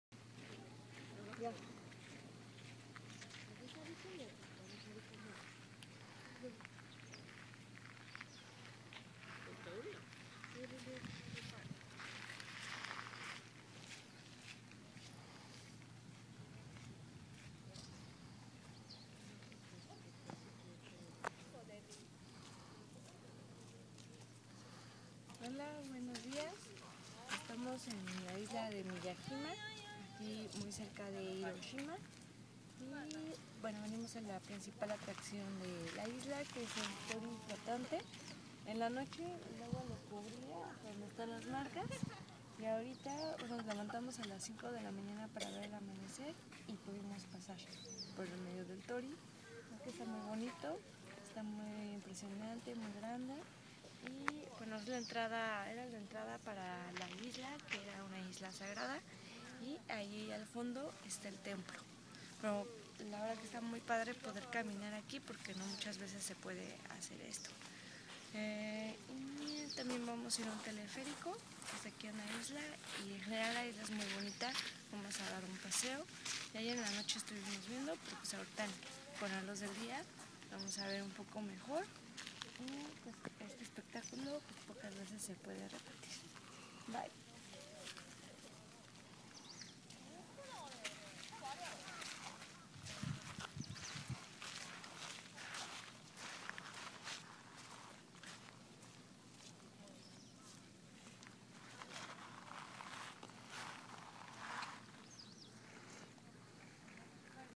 Viaje a Hong Kong y Japon --- TTT trip --- Miyajima Japón
En este video puedes ver el Tori sagrado de la isla de Miyajima en Japón, es impresionante verlo al amanecer, con  su color rojo característico. En la mañana la marea esta baja y puedes caminar y pasar por en medio de el. Al medio de día el agua vuelve a subir y solo se puede pasar en lancha.